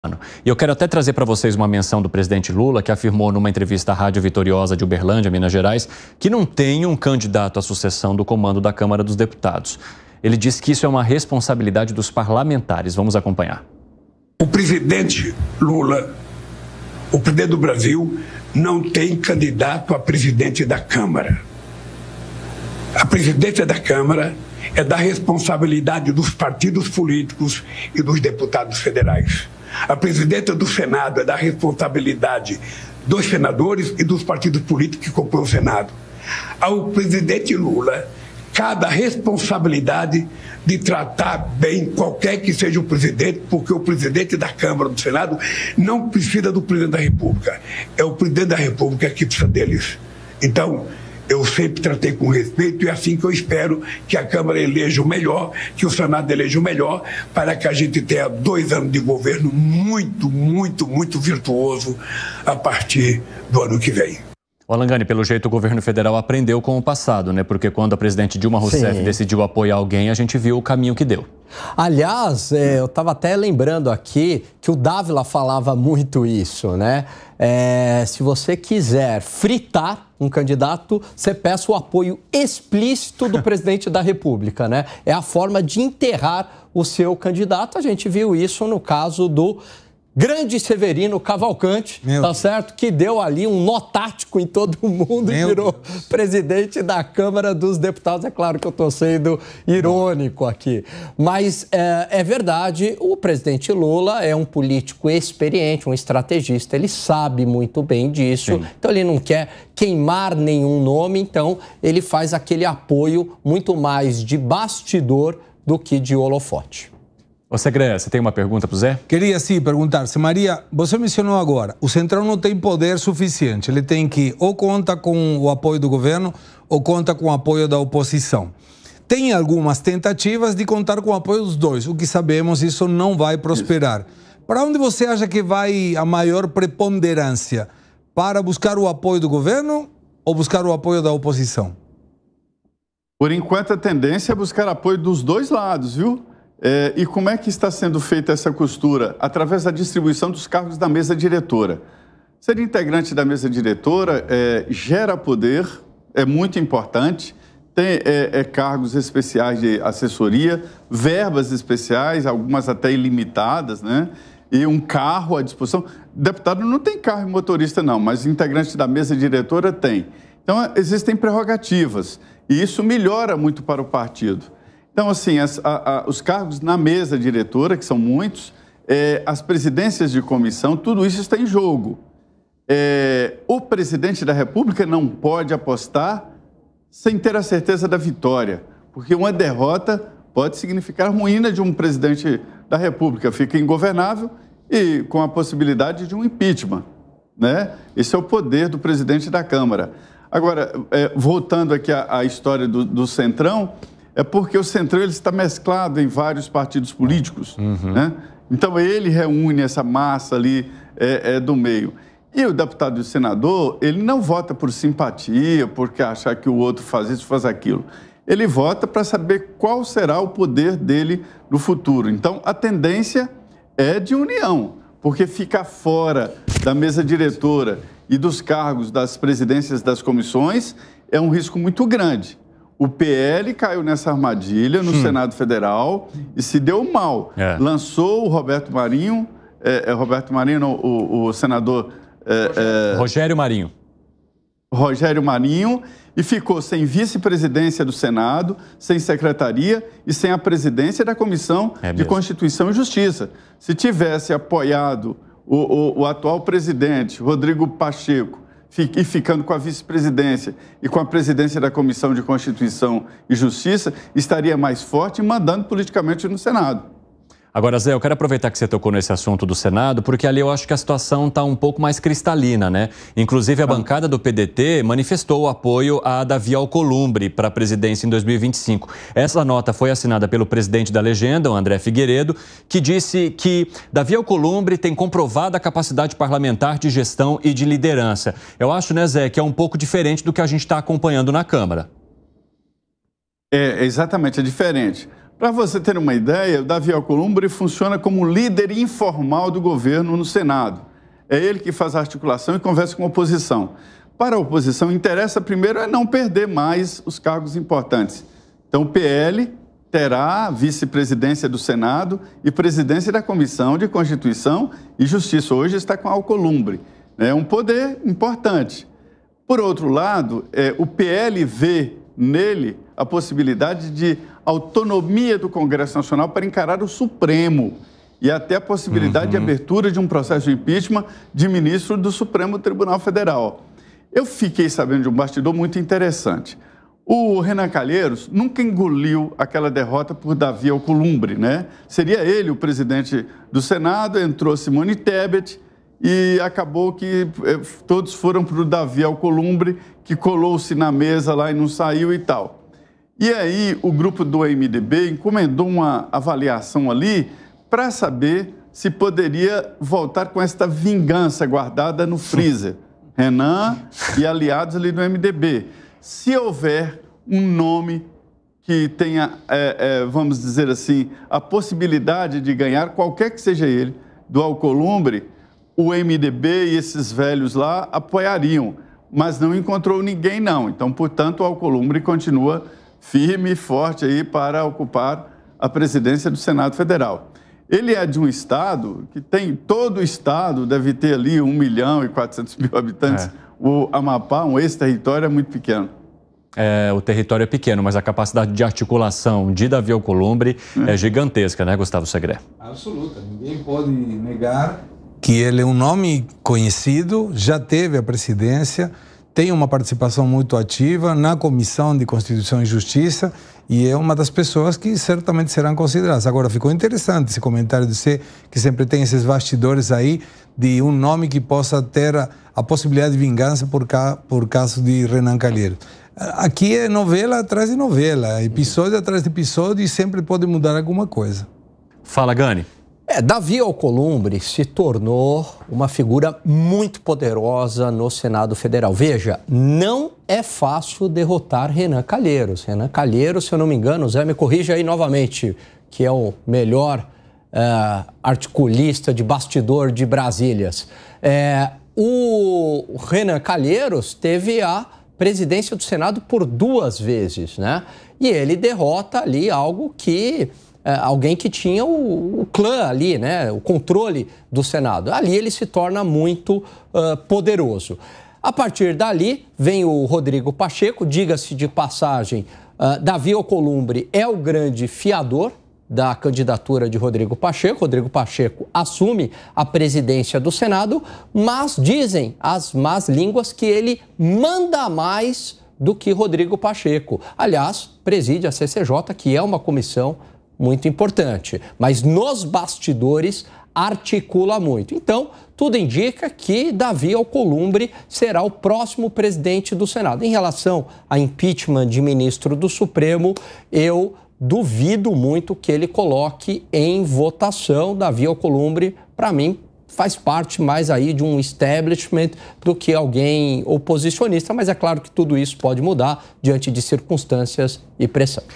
Lula: “Não tenho candidato a presidente da Câmara”; Trindade, Ghani e Segré comentam
O presidente Lula (PT) declarou que não possui um candidato para suceder Arthur Lira (Progressistas) na Presidência da Câmara dos Deputados, ressaltando que a escolha é uma responsabilidade dos partidos políticos e dos deputados. Além disso, o PDT anunciou apoio a Davi Alcolumbre para a presidência do Senado.